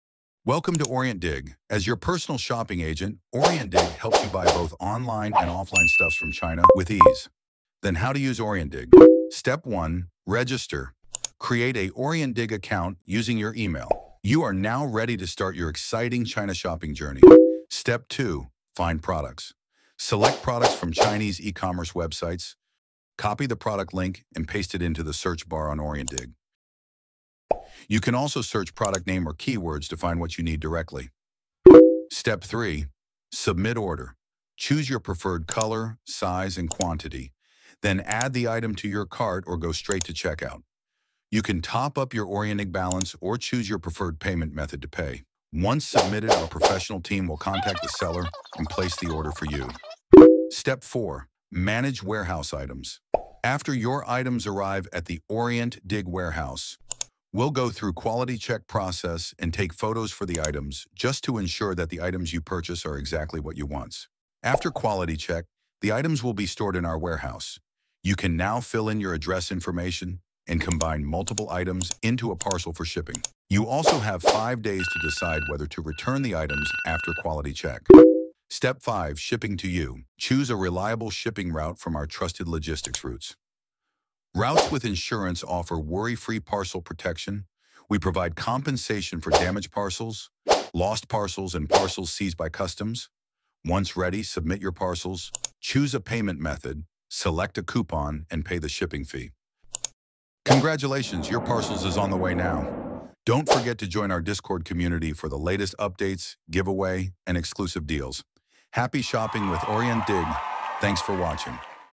How to use OrientDig
A brief guide of how to use OrientDig to buy products from Taobao/Weidian/1688 etc.